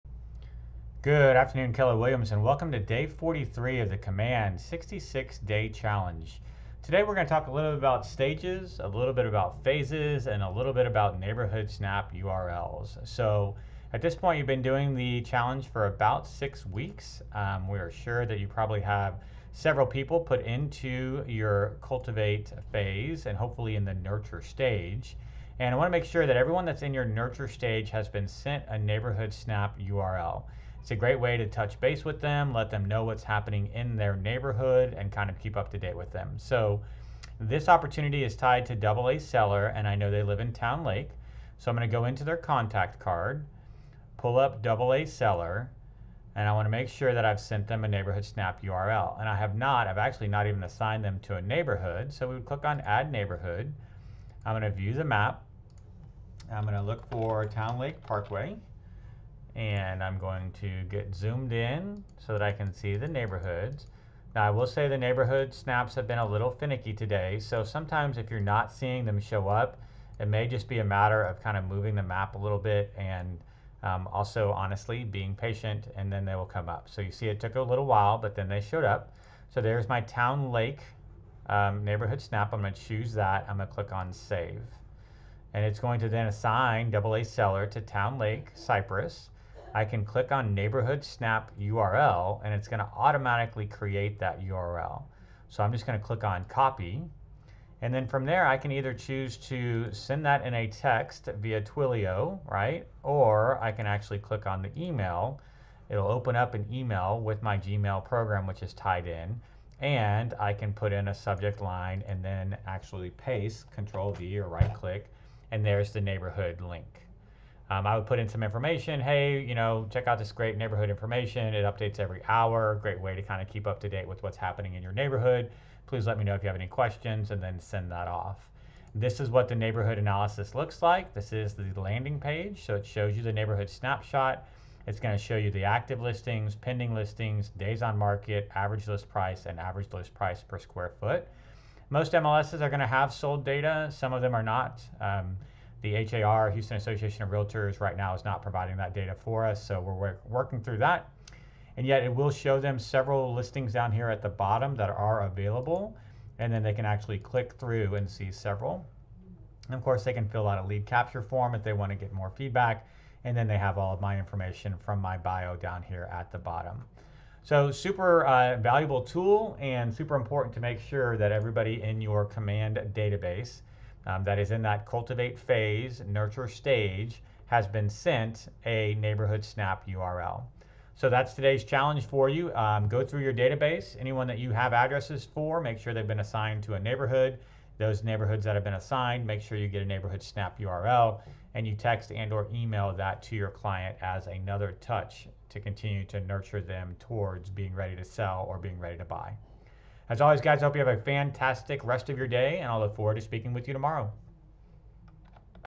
Keller Williams Command 66 Day Challenge 43 - Nurture Neighborhood Snaps
Challenge Day Forty Three - Six weeks in, your Keller Williams Command database should be filling up with Contacts and those Contacts placed in Opportunity/Sales Pipeline.  While we're always cognizant of our Hot Contacts, it's those that we're attempting to Nurture that sometimes fall through the cracks.  Today's Challenge offers a great way to reach out and touch those Nurtures with a Neighborhood Snap URL.

Watch this short video and then ensure that all of your Contacts in the Cultivate/Nurture Phase/Stage of your Sales Pipeline have been assigned to a neighborhood.  Next, create a Neighborhood Snap URL and send it to each of these Contacts as an additional touch point.

Once you've done so, comment "Done." below.